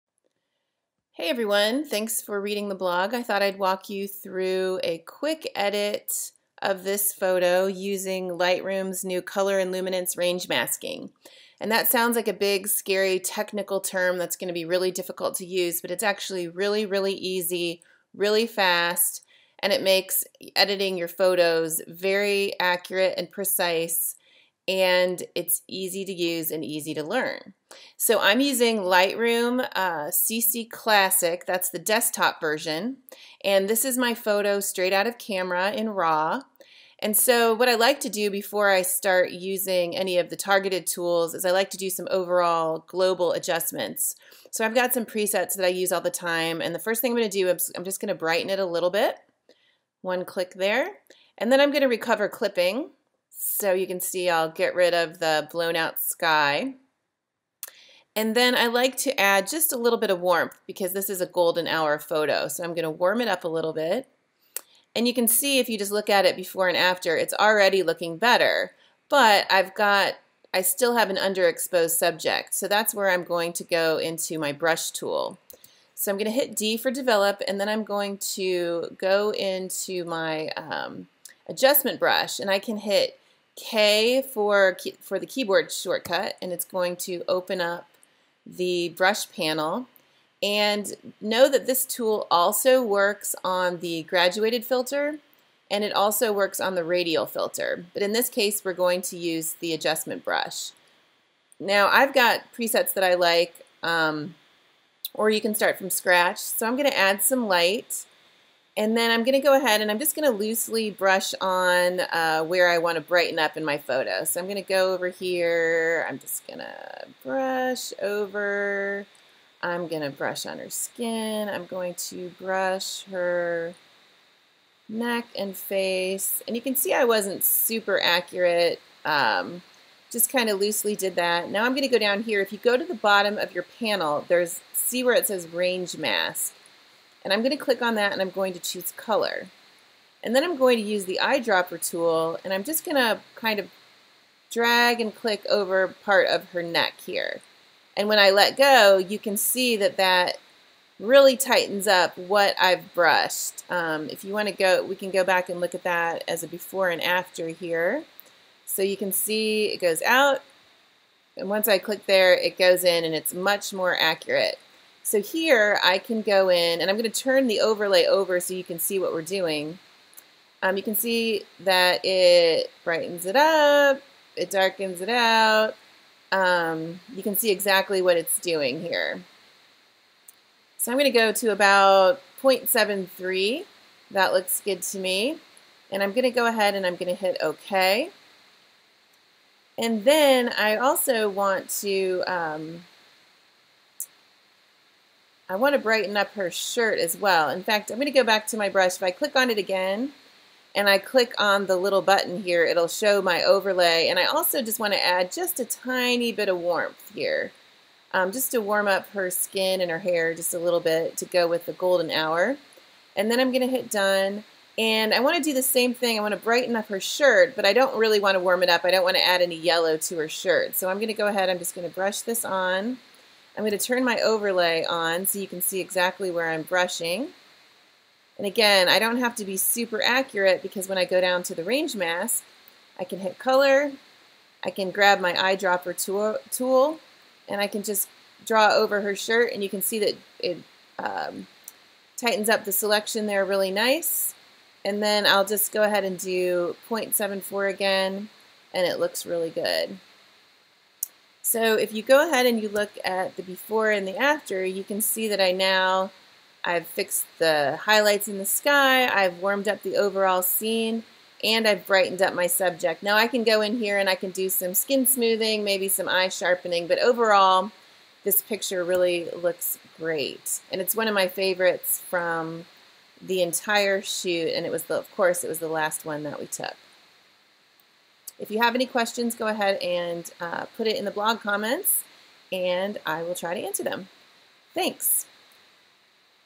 Lightroom CC Classic Range Mask Tool Demo
Use the new color range masking tool in Lightroom CC Classic to easily brighten a backlit subject.